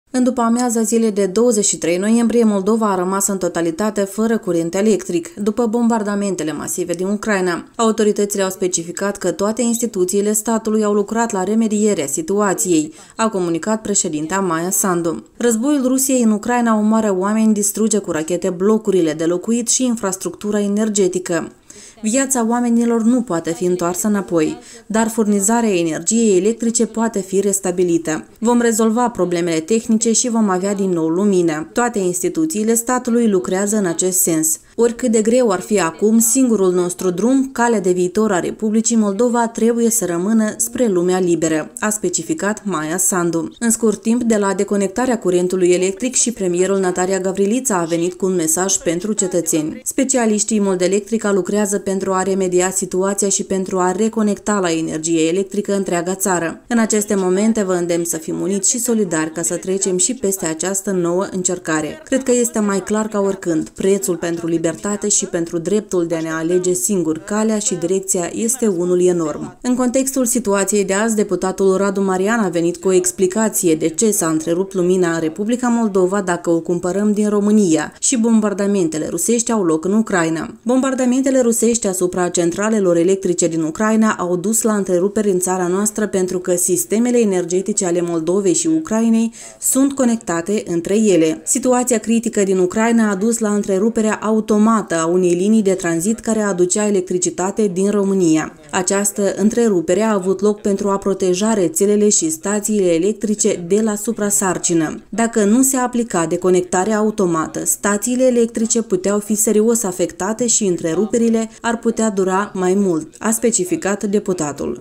MOLDOVA A RĂMAS ASTĂZI FĂRĂ LUMINĂ
În după amiaza zilei de 23 noiembrie, Moldova a rămas în totalitate fără curent electric, după bombardamentele masive din Ucraina. Autoritățile au specificat că toate instituțiile statului au lucrat la remedierea situației, a comunicat președinta Maia Sandu.
”Războiul Rusiei în Ucraina omoară oameni, distruge cu rachete blocurile de locuit și infrastructura energetică. Viața oamenilor nu poate fi întoarsă înapoi. Dar furnizarea energiei electrice poate fi restabilită. Vom rezolva problemele tehnice și vom avea din nou lumină. Toate instituțiile statului lucrează în acest sens… Oricât de greu ar fi acum, singurul nostru drum, calea de viitor a Republicii Moldova, trebuie să rămână spre lumea liberă.”, a specificat Maia Sandu.